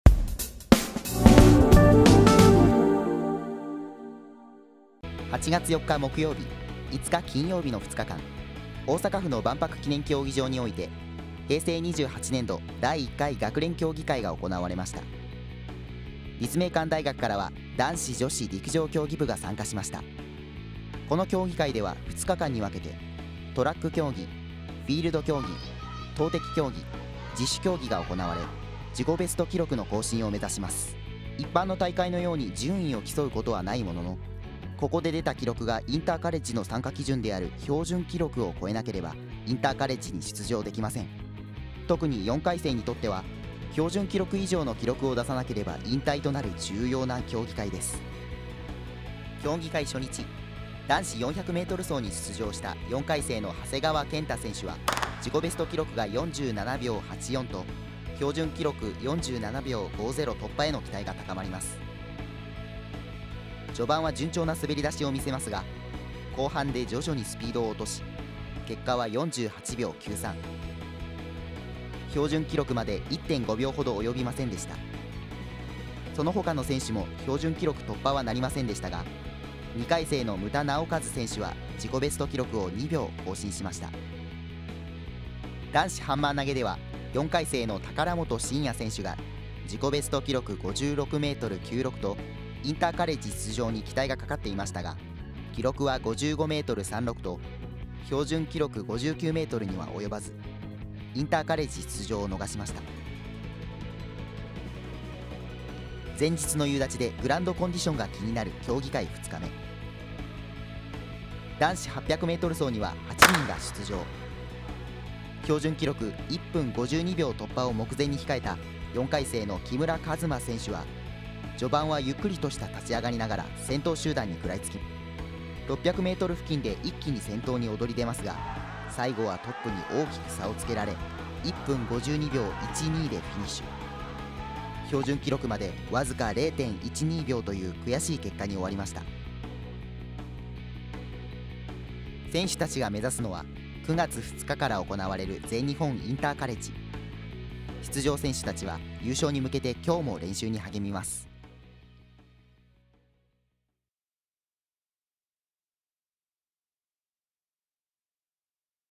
陸上競技部第1回学連競技会【2016年8月4日・5日】
2016年8月4日、5日万博記念陸上競技場にて、陸上競技部が第1回学連競技会に出場しました。この競技会はインカレへと進むための重要な会で、選手たちは記録突破を目指し、奮闘していました。